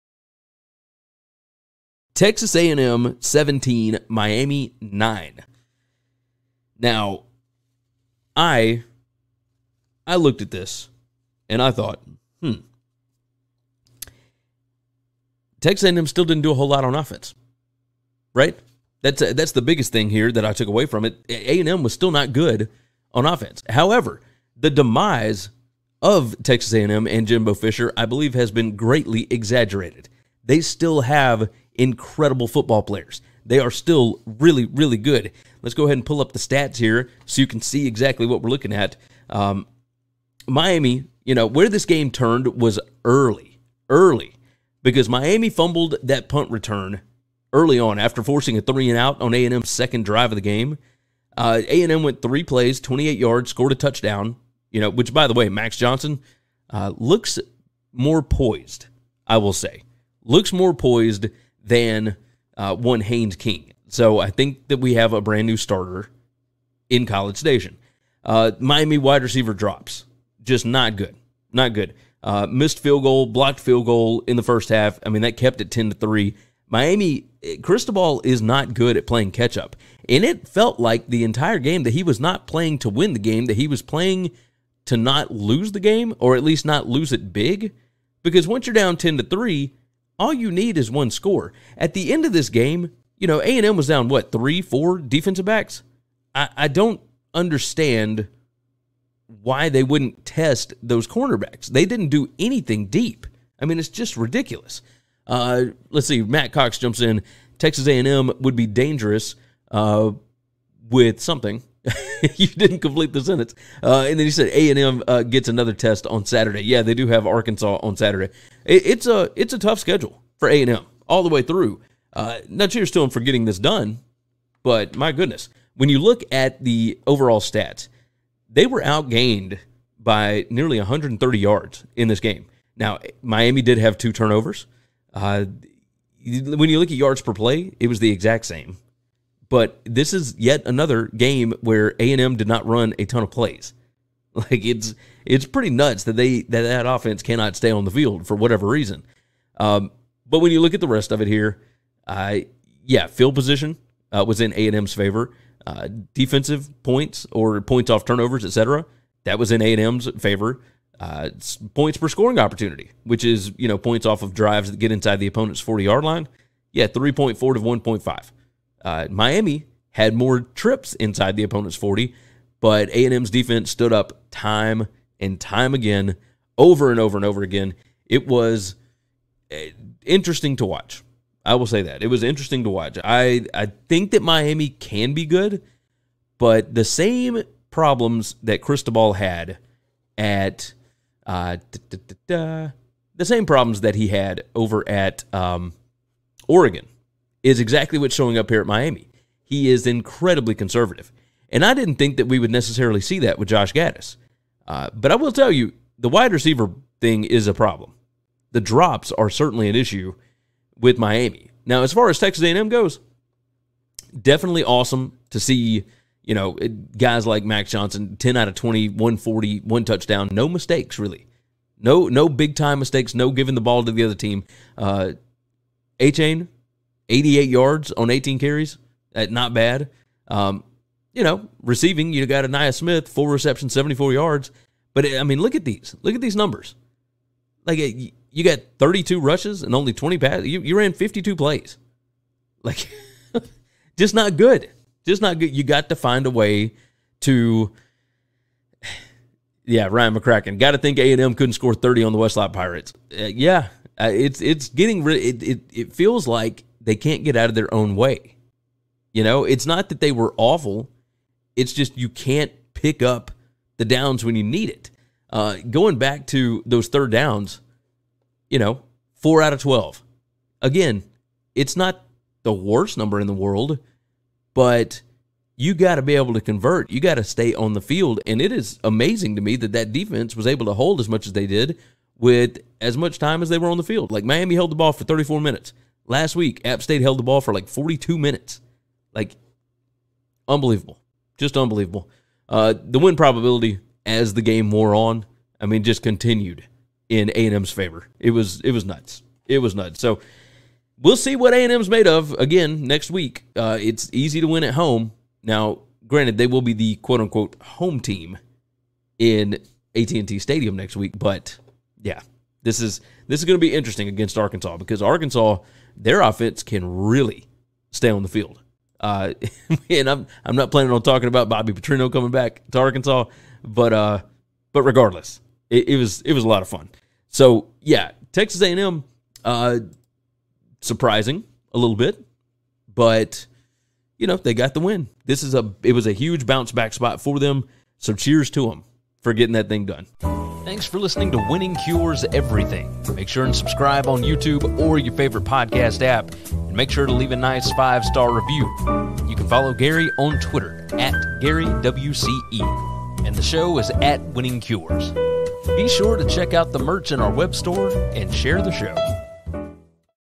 Texas A&M vs Miami quick reaction & recap College Football 2022
Gary gives his quick reaction, breakdown & recap to Texas A&M’s big win vs Miami in Week 3 of the 2022 College Football season.  What do we make of Texas A&M coach Jimbo Fisher or Miami coach Mario Cristobal after this one?  What happened here, what do the stats say, what’s the post game win expectancy?